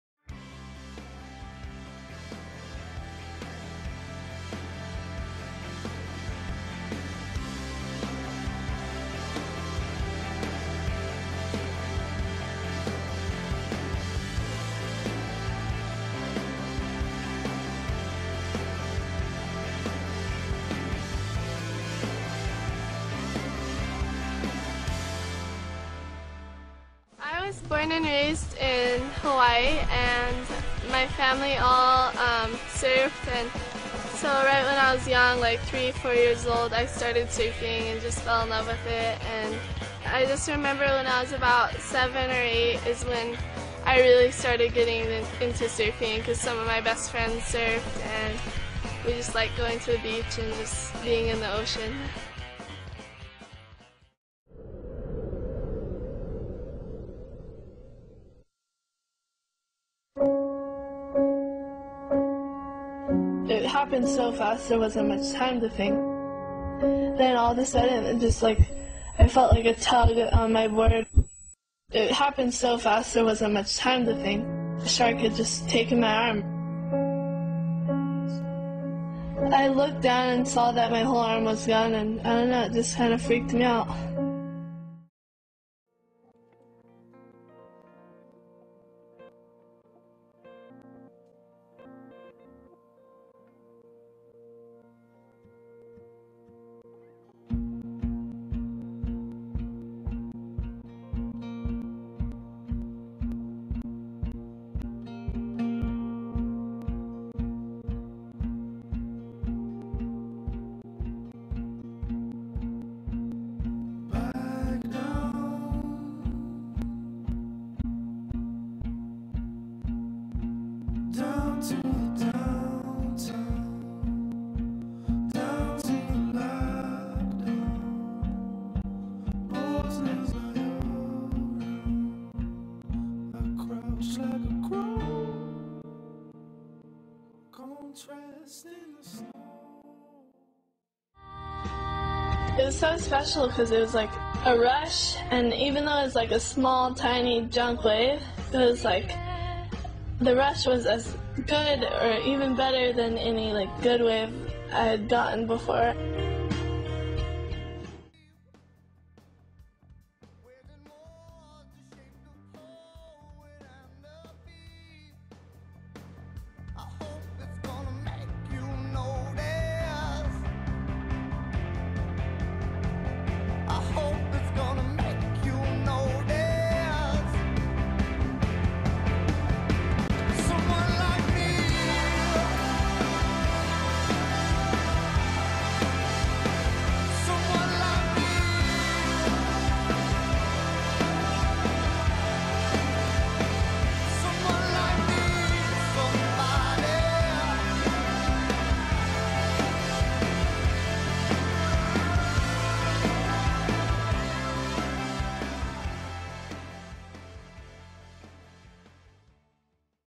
Bethany Hamilton
This is a photo essay done at UTS for Documentary production by Sara Ringhagen. It is stuff from youtube edited together to tell the story.